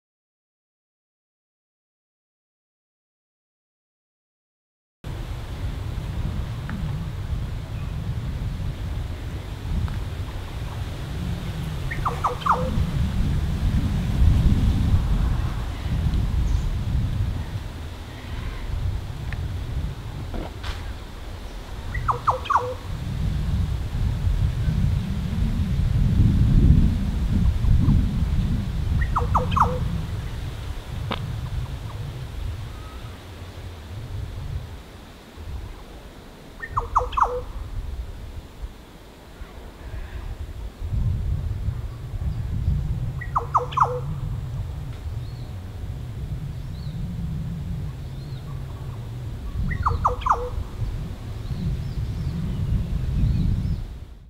Green Oriole (Oriolus flavocinctus) - Birds of Australia
A terrible video of a Green Oriole (Oriolus flavocinctus) at Cattana Wetlands in November, 2016. It was a great view of the bird, but the day was very windy, hot, and my video is pretty awful. It does capture the lovely, watery call, though.